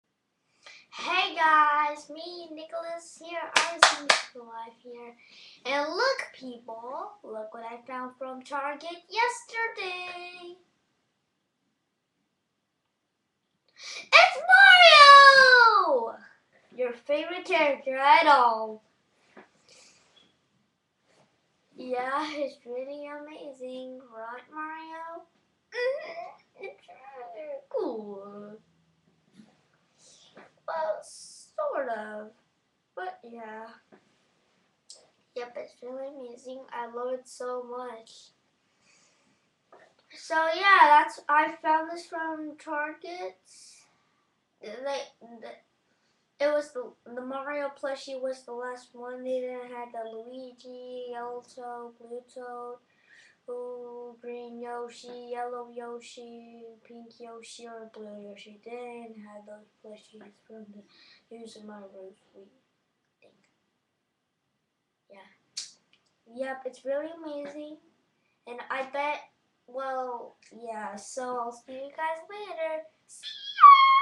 look what I found @ target yesterday
LOOK @ DESCRIPTIONS

I hade the last one so they didn't hade luigi,yellow toad,blue toad,green yoshi,pink yoshi,yellow yoshi,or blue yoshi and umm yeah i loved the mario and I will talk to you later bye and SUBSCRIBE!